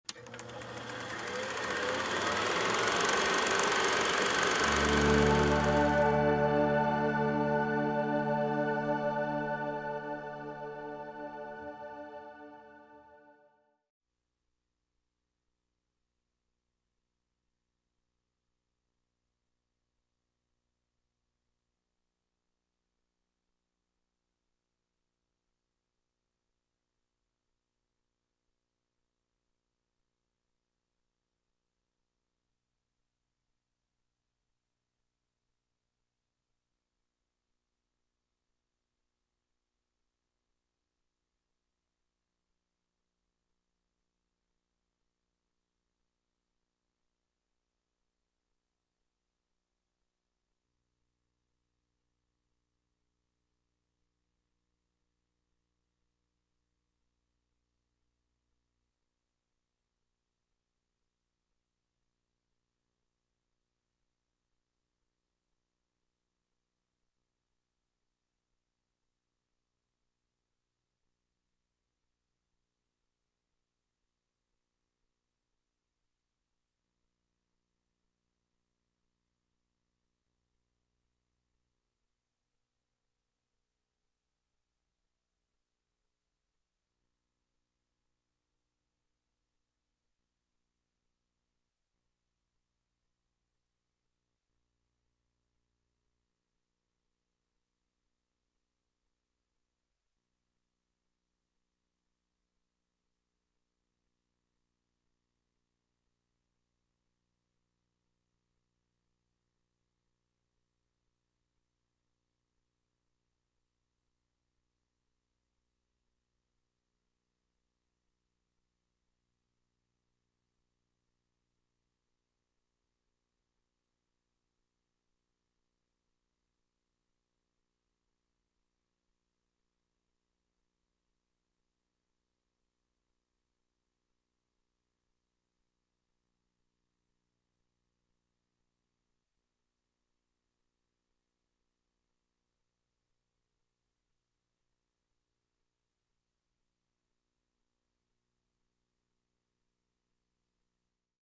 Atlanta Negro Chamber of Commerce Film Collection: Clip 1
This video clip is from a series of films commissioned by the Atlanta Negro Chamber of Commerce founder in ca. 1938.  The entire series documents a variety of activities primarily in the African American communities in the Atlanta area including social events, sporting activities, housing developments, and work place activities.  This particular clip focuses on sporting and social events. The entire feature is available for viewing in the research room of the Kenan Research Center at the Atlanta History Center.